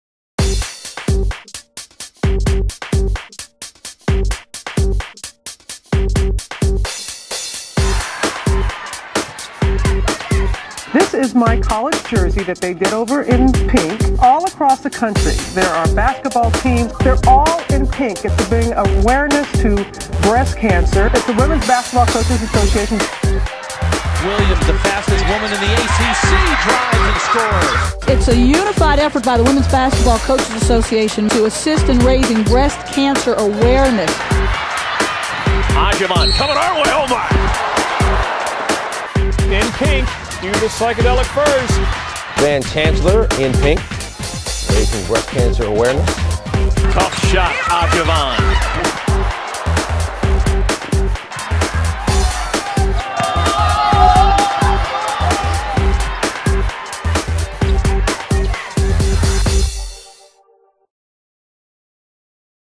WBCA Pink Zone Promo
Join the Alabama A&M basketball teams in raising awareness for cancer research with Pink Zone night on Saturday, Feb. 20 vs. Southern. Women tip at 4:00 with the men to follow at 6:00.